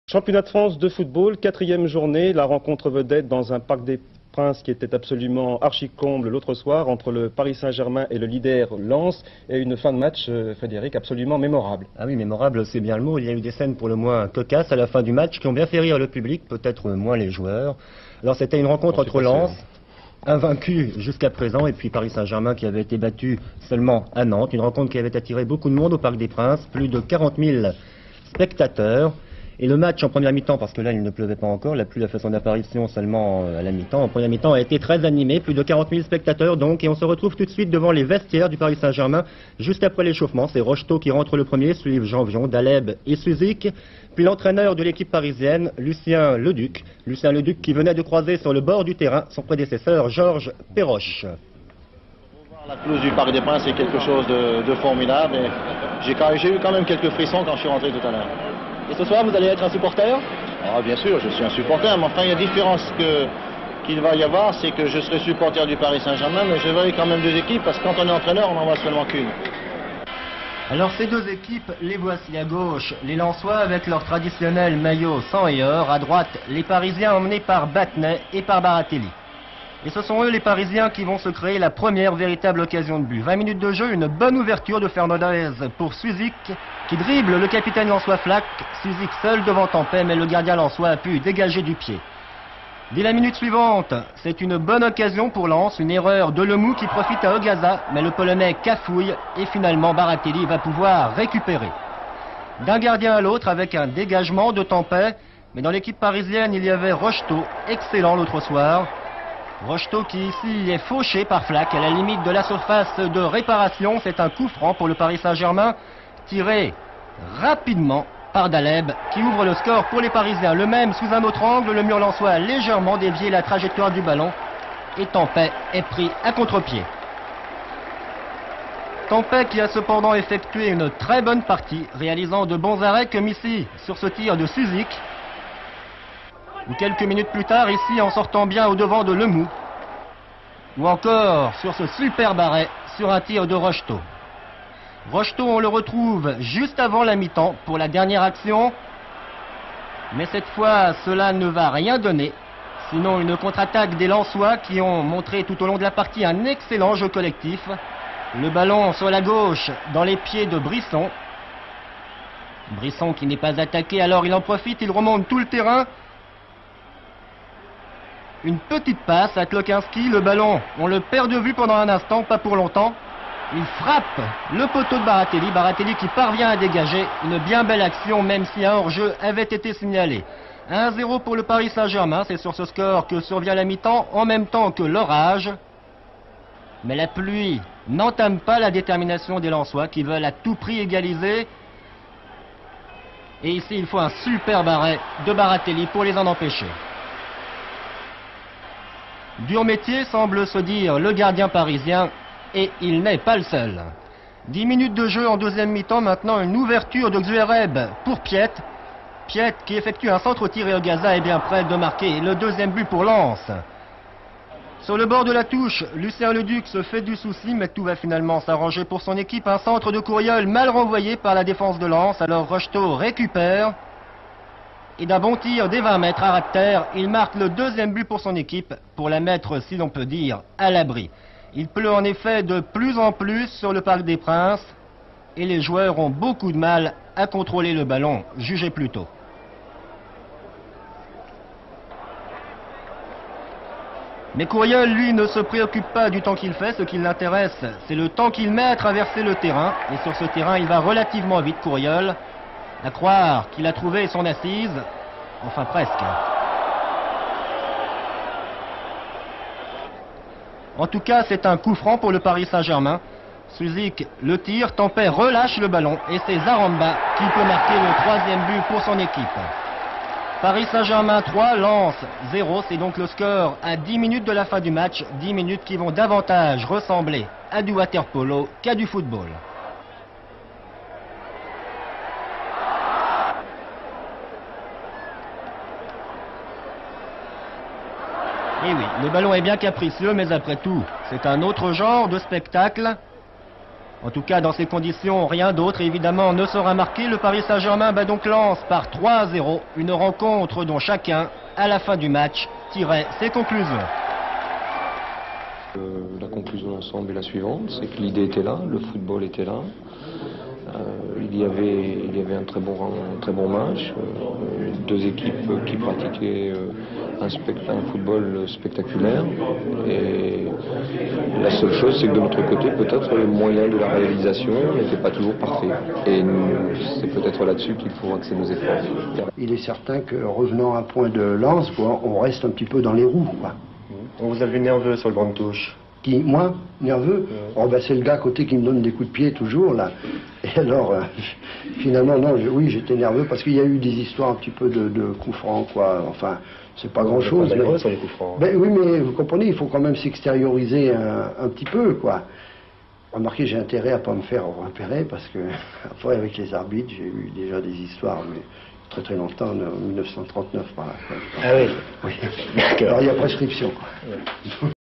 [Résumé] 10/08/1983 - Division 1 - Journée 04 - PARIS SG LENS 3 - 0
Paris SG - Lens Lourde défaite au parc !

Buts Parisien :
- Mustapha Dahleb (23)
- Dominique Rocheteau (68)
- Pascal Zaremba (79)